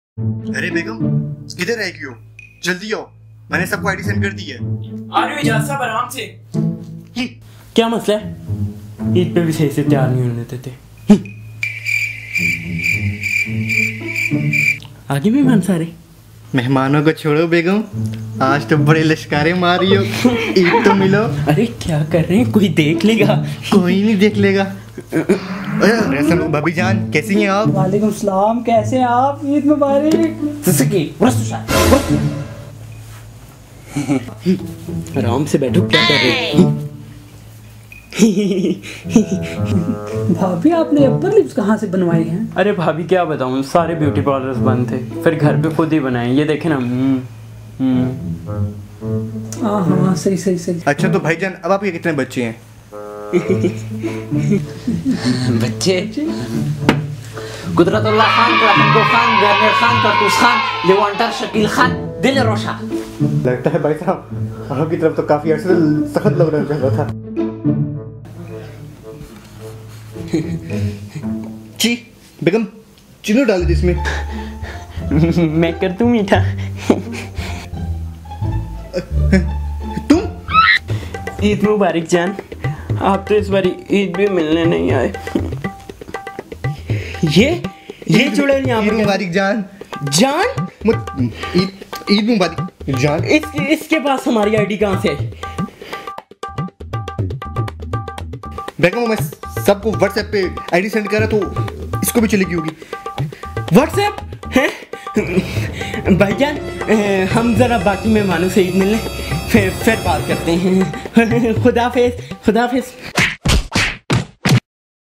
Online Eid | Funny Skit | Eid Special |Bachelor Bois
Online Eid will be like this. Tell us your favorite scene in comments. :')

-M.sameer.2002
-usmanshafiq958
-umertariq
-Bachelorbois

If you are reading This. We love you ❤️